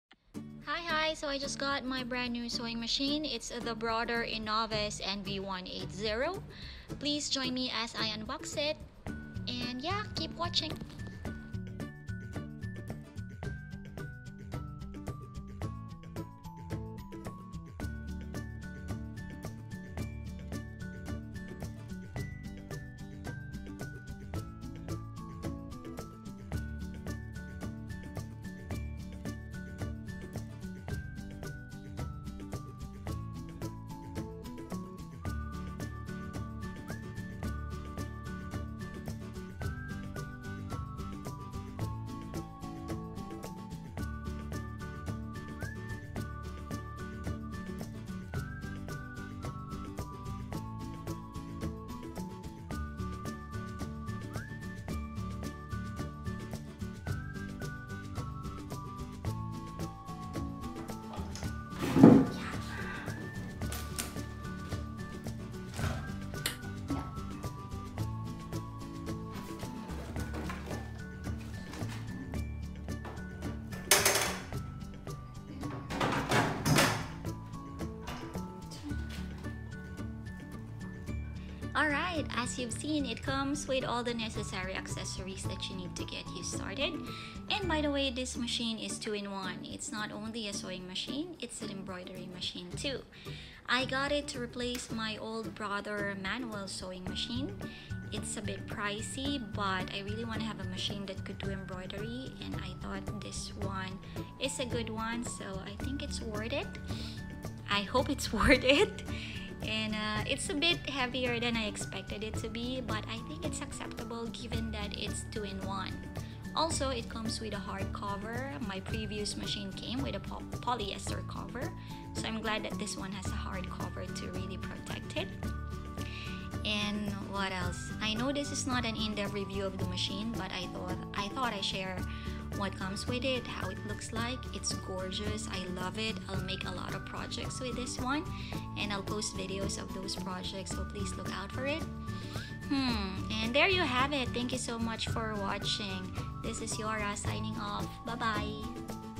Brother Innov-is NV180
Just got my hands on NV180. Unboxing video guys!